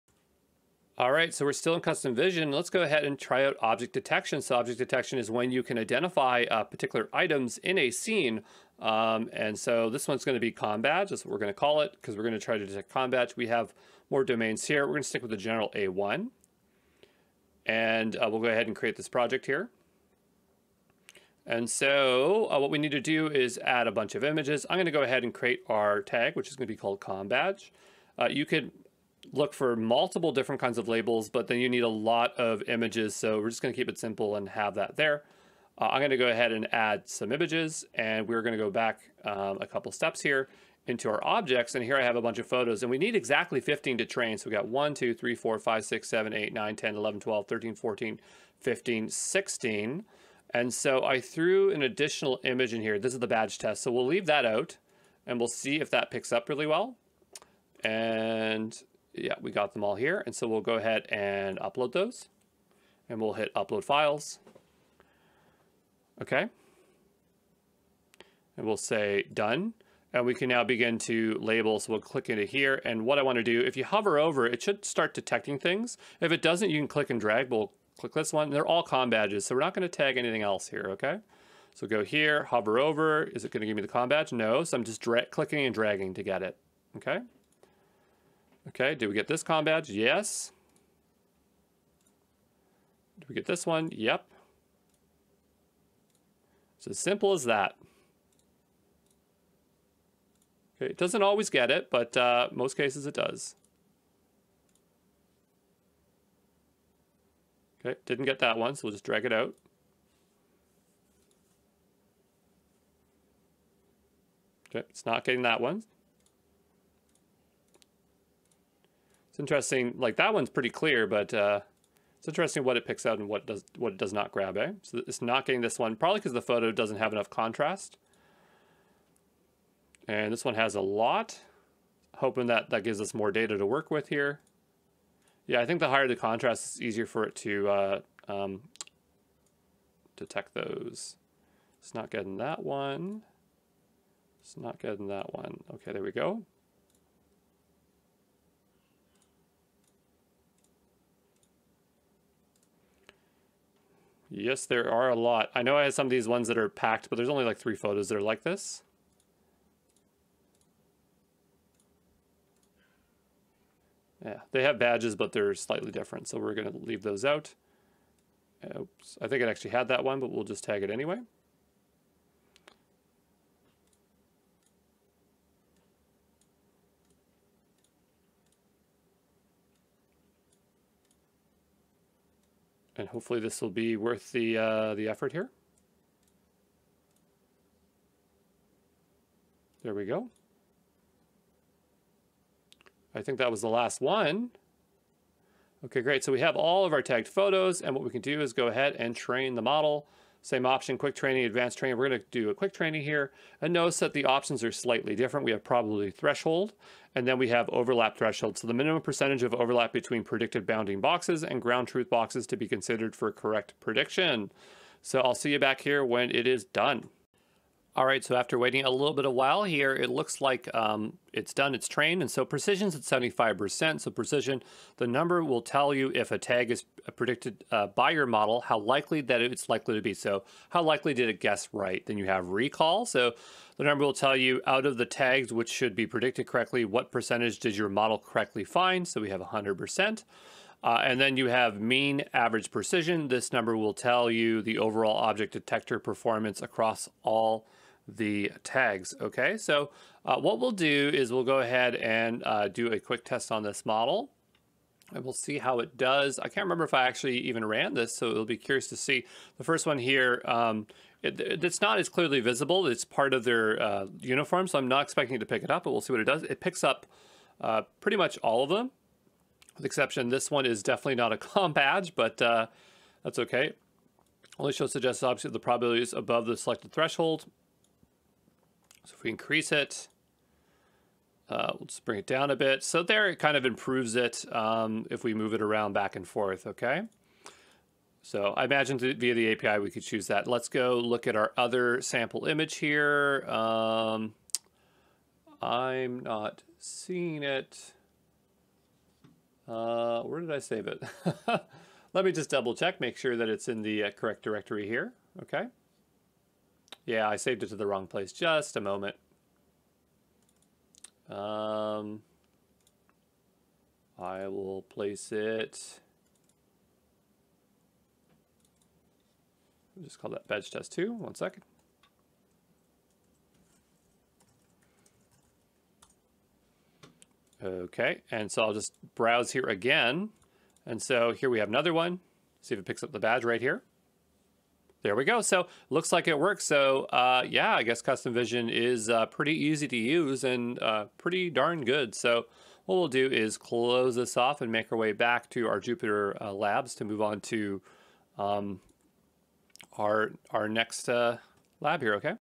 Custom Vision Object Detection - Azure AI Course
Learn about object detection using Custom Vision in this practical video. Understand how to identify and locate objects within images.